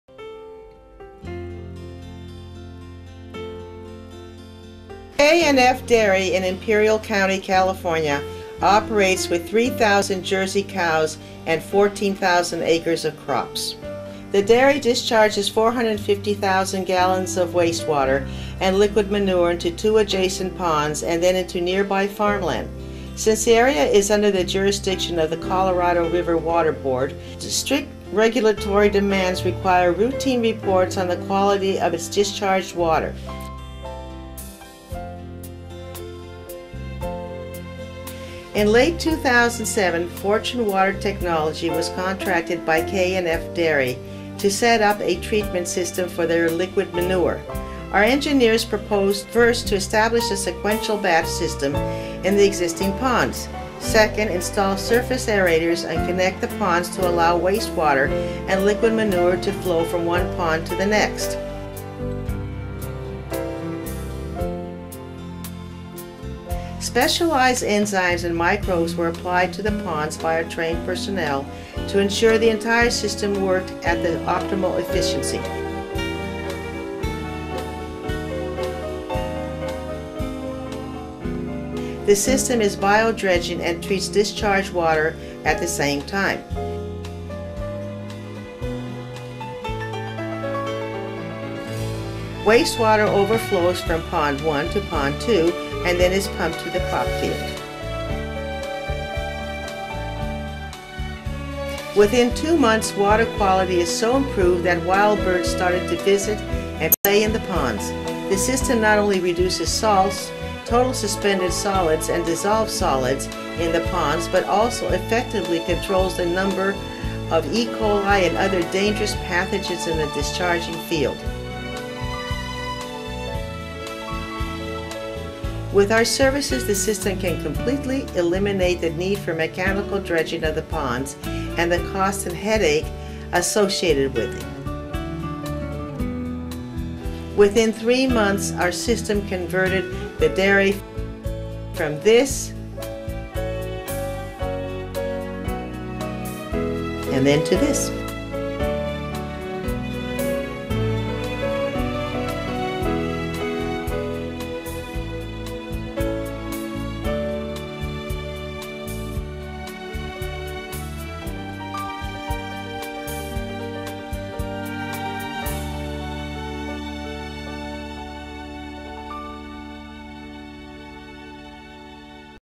Dairy Farm Bio-Dreding & Waste Water Treatment
Solving liquid cow manure problems once and for all with bio-dredging. Own an on-site treament systems inexpensively. ECO friendly. Reduce salt and E-coli.